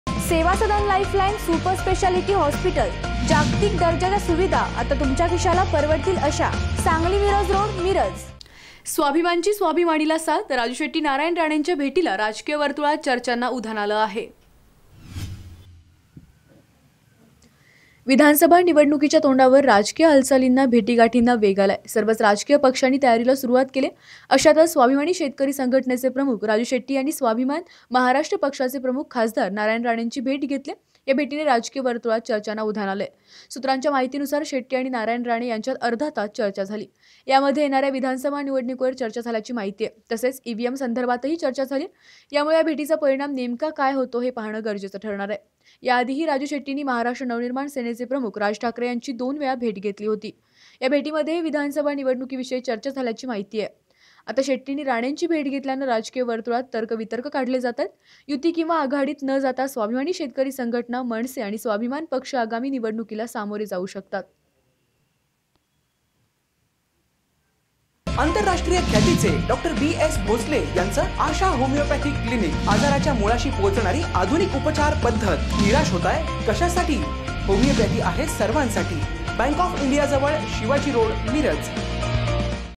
स्वाभिमान'ची 'स्वाभिमानी'ला साथ? राजू शेट्टी नारायण राणेंच्या भेटीला
स्वाभिमान'ची 'स्वाभिमानी'ला साथ? राजू शेट्टी नारायण राणेंच्या भेटीला, राजकीय वर्तुळात चर्चांना उधाण.@